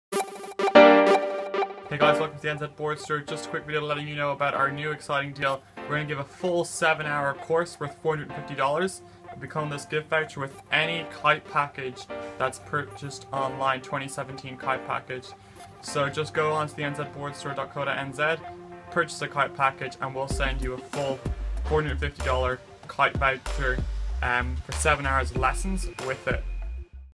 Free Kitesurfing Lessons With 2017 Kite Packages
We are offering a Free 7hr Kitesurfing Course with 2017 Kite Packages.